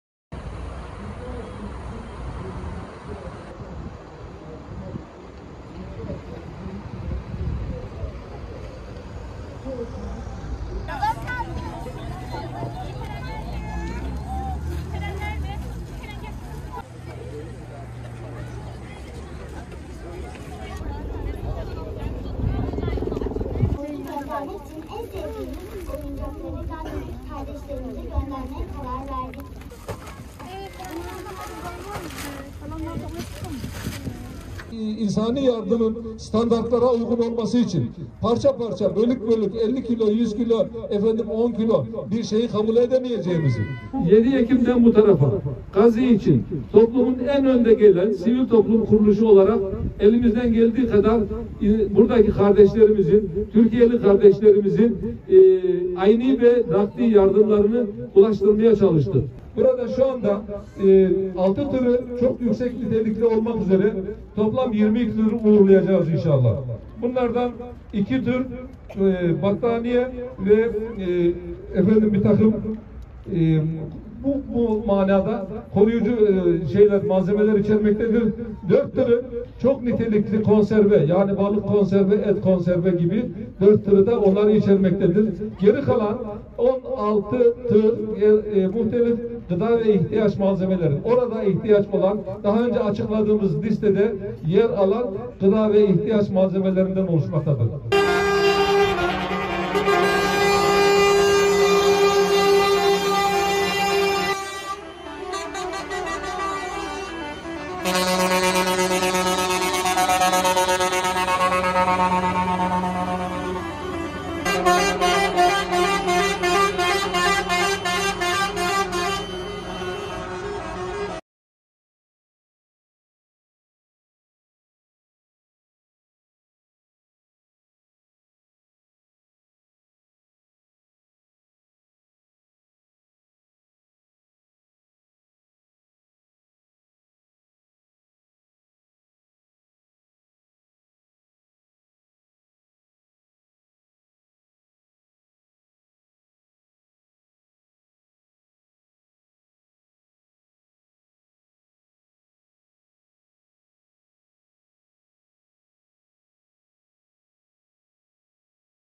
Konya'dan Gazze'ye 22 yardım tırı gönderildi
Konya İnsan Hak ve Hürriyetleri (İHH) İnsani Yardım Derneği tarafından, "Özgürlük Filosu Yeniden Yola Çıkıyor" kampanyası kapsamında toplanan gıda ve çeşitli ihtiyaç malzemeleri yüklü 22 tır, Gazze'ye uğurlandı.